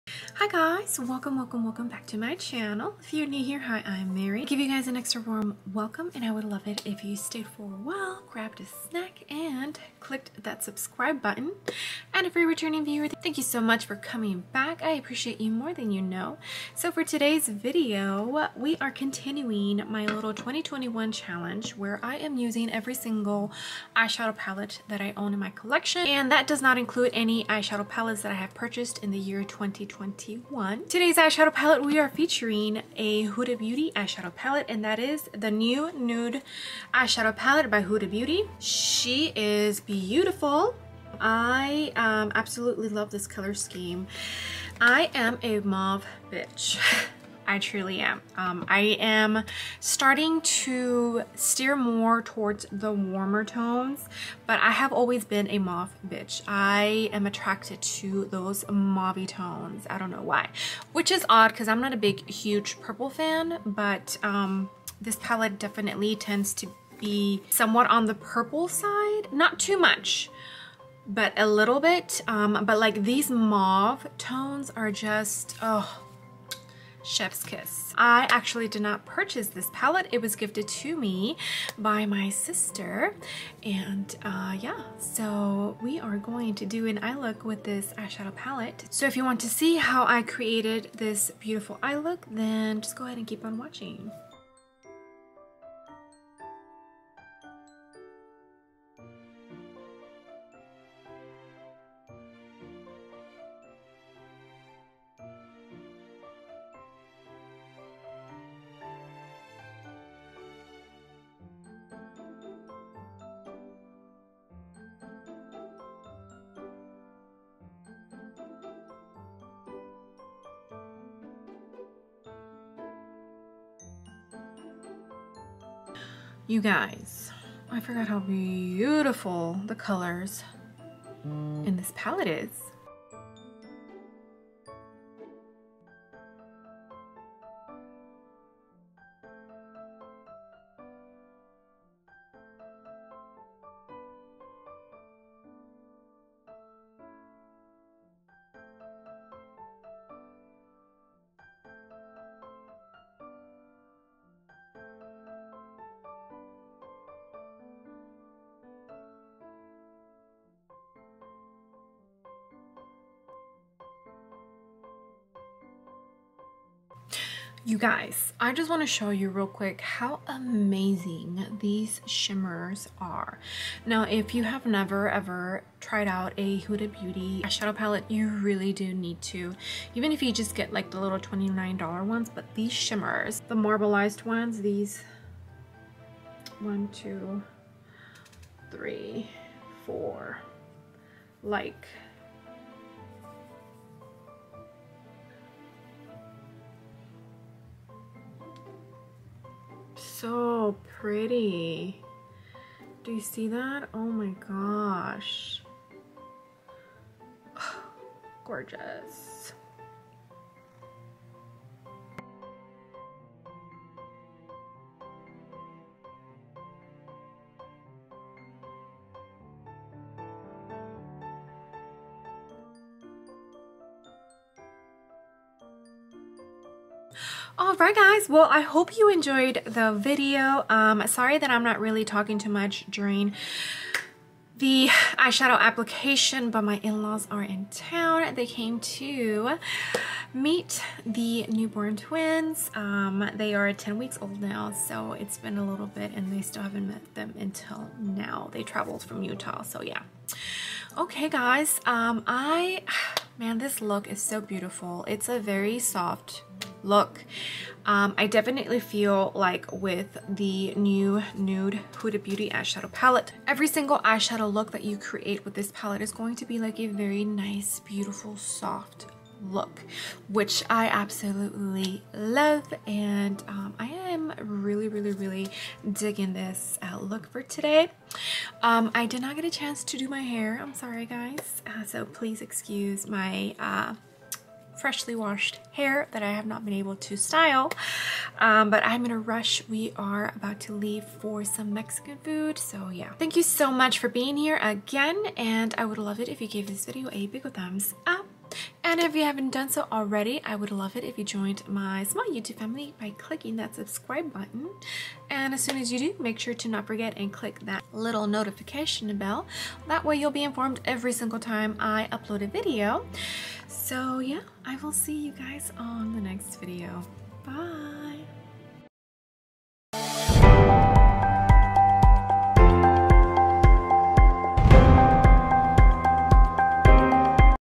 2021 CHALLENGE | palette #5 | Huda Beauty New Nude eyeshadow palette
Hello lovelies! It’s time for another eyeshadow look for my 2021 challenge 😀 this is definitely in my top 10 eyeshadow palettes, it’s just such an amazing palette. If you love mauvey tones this palette is for you ❤️

Outro by Manique Here ⬇️